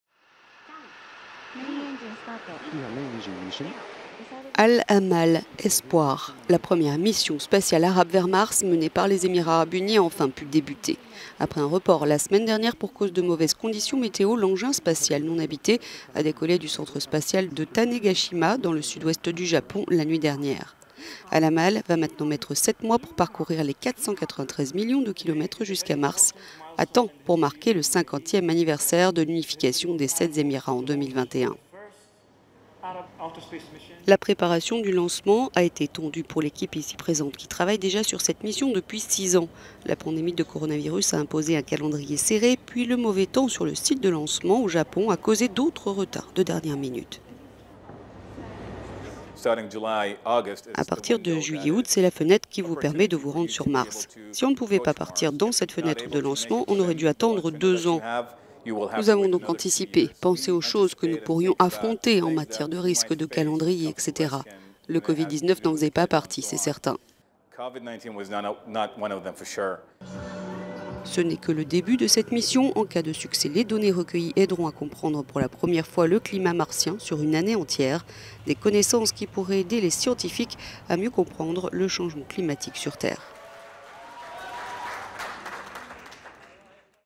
La sonde Al-Amal en route pour Mars, c'est la première mission arabe dans l'espace
🛰️ Cette mission, la première entreprise par les Émirats arabes unis, doit nous en apprendre plus sur le climat de la planète rouge